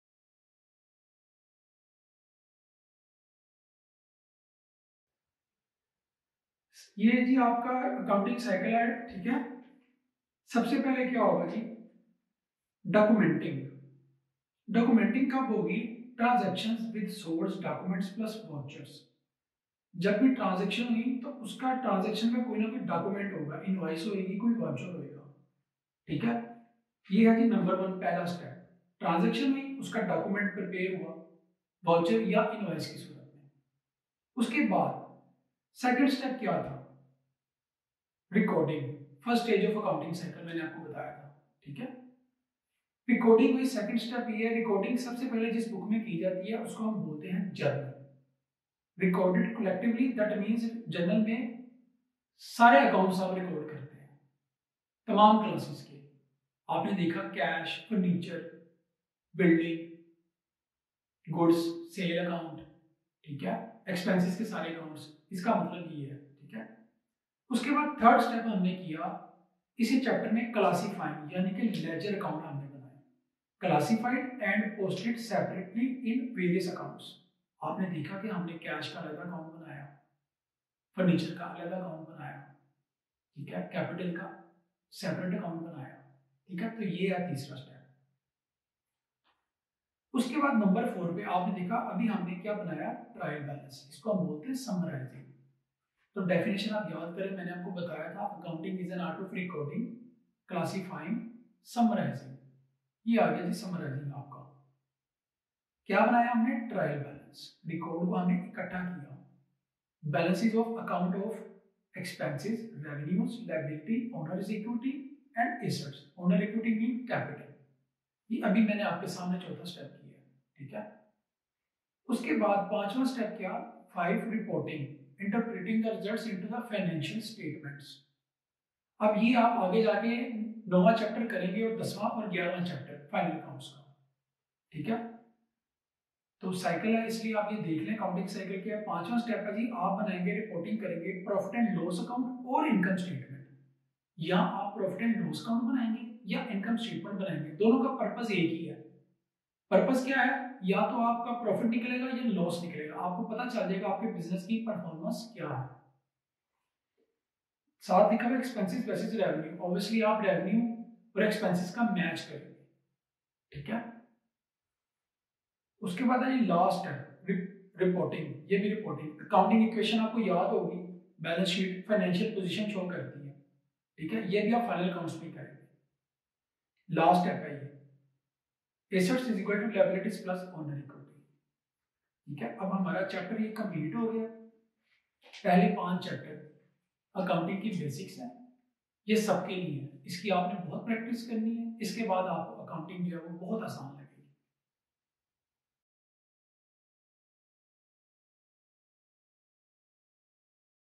Financial accounting | I.com ( first year ) | Chapter No 5 : Ledger: Main accounts | Lecture No 48
Here are the complete video lectures of I.com first-year financial accounting
Chapter No 5: Ledger: The main books of accounts
Lecture No. 48   Discussion about the complete accounting cycle